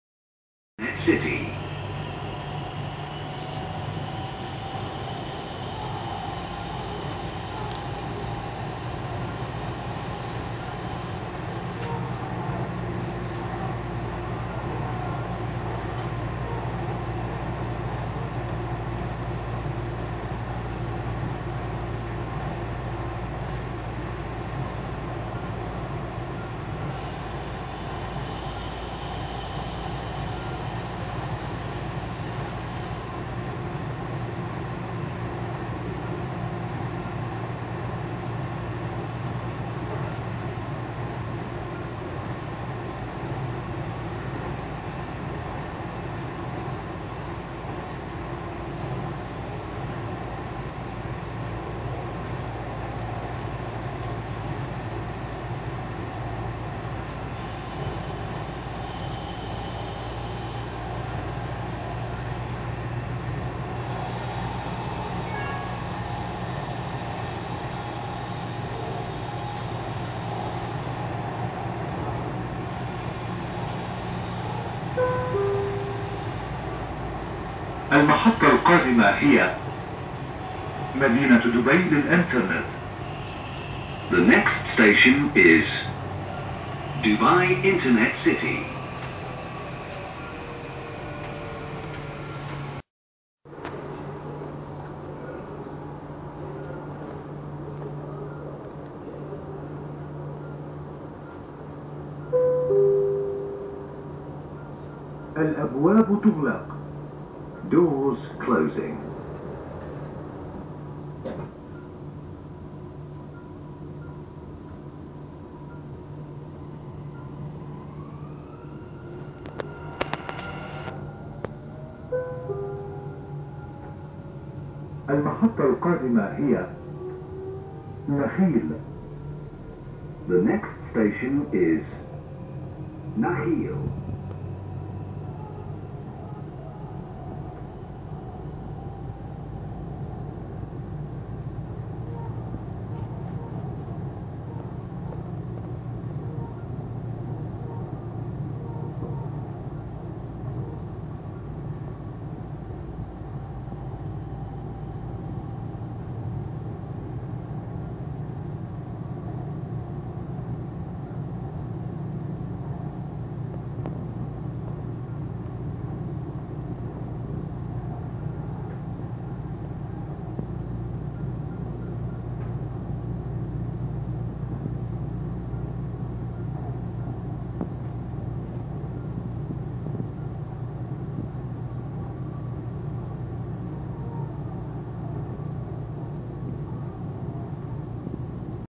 Train Ride to Work
The last few stops to show at which building our office is located.